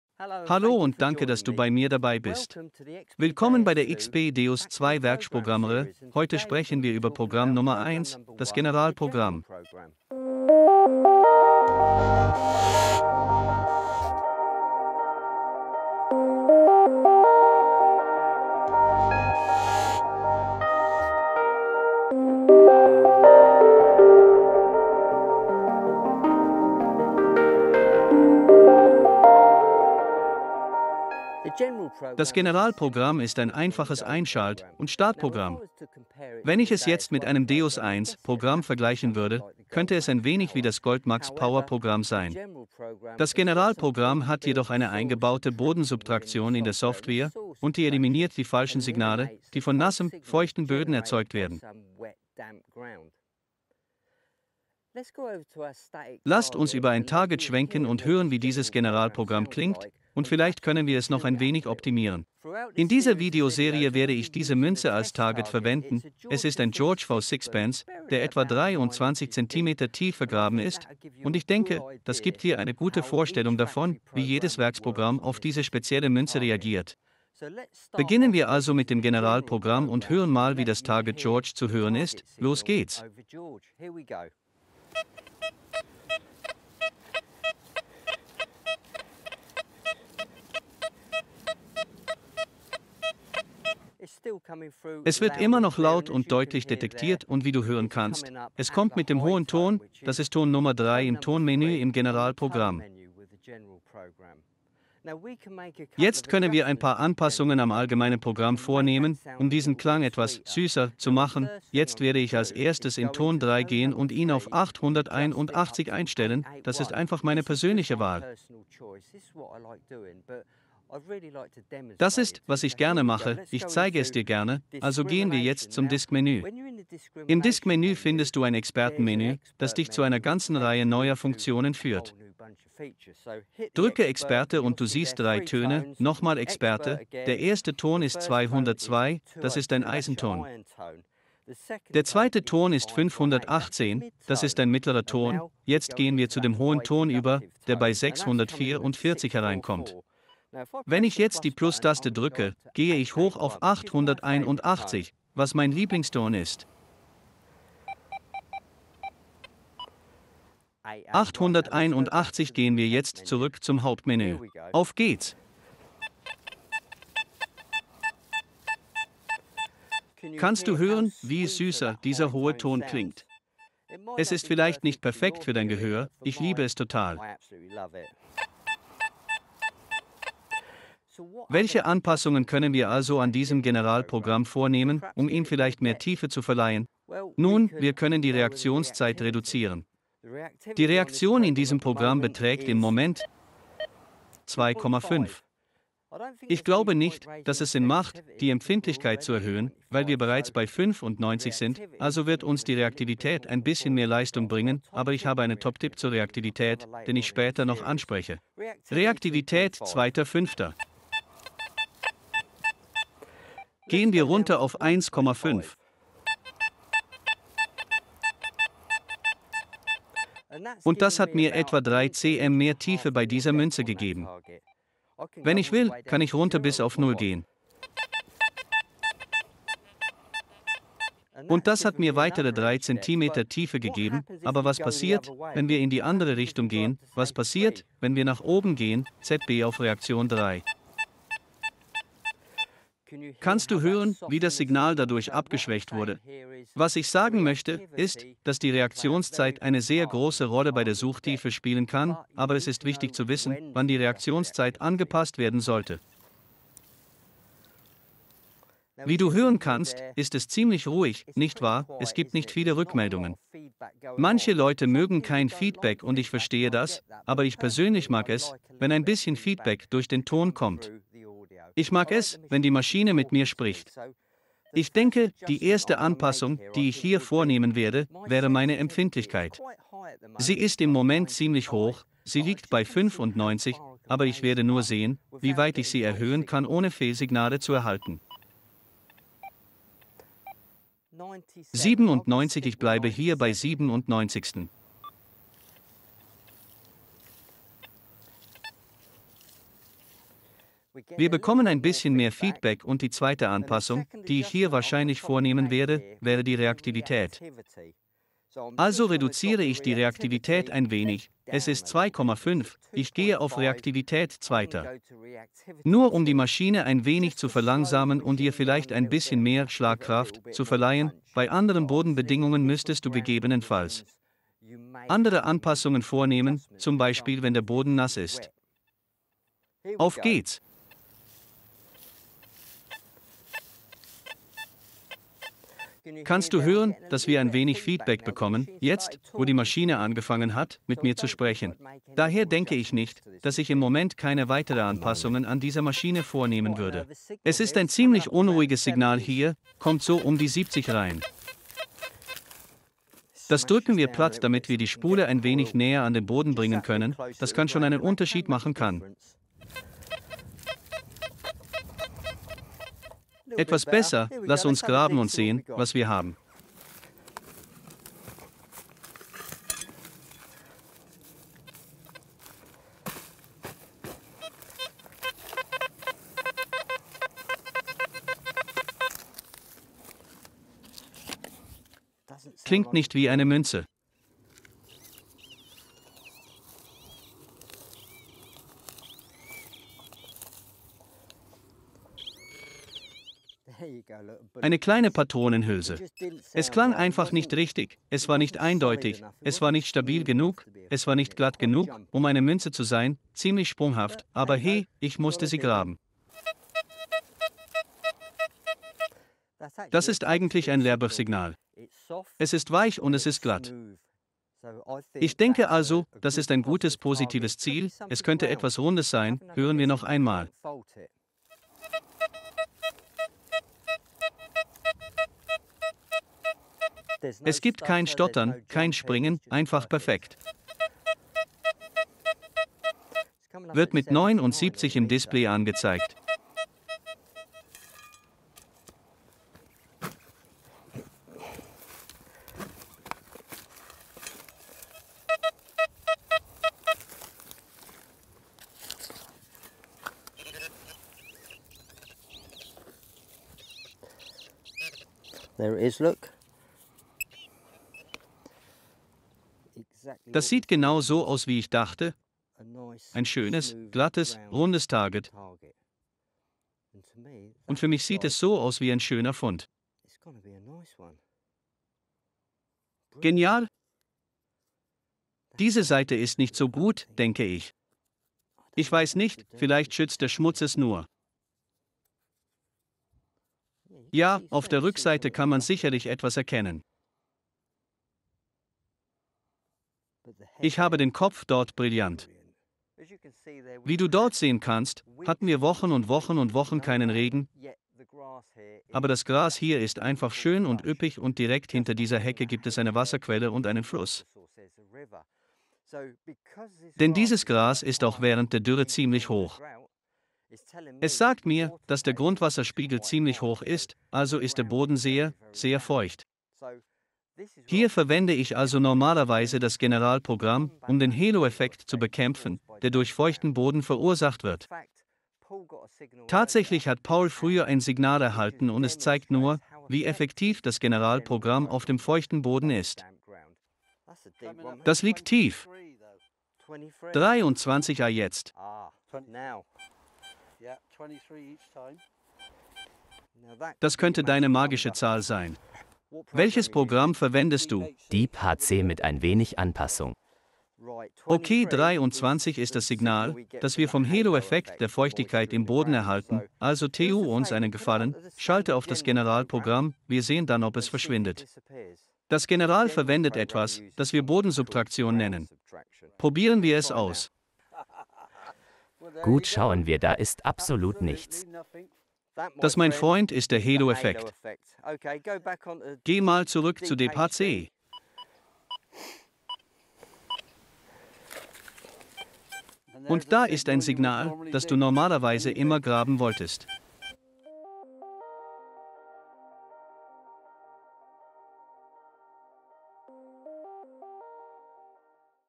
XP Metalldetektor Deus II - General Programm 1 erklärt
Garry erklärt euch das XP DEUS II General #1 Programm

Weitere Videos von Garry folgen ;)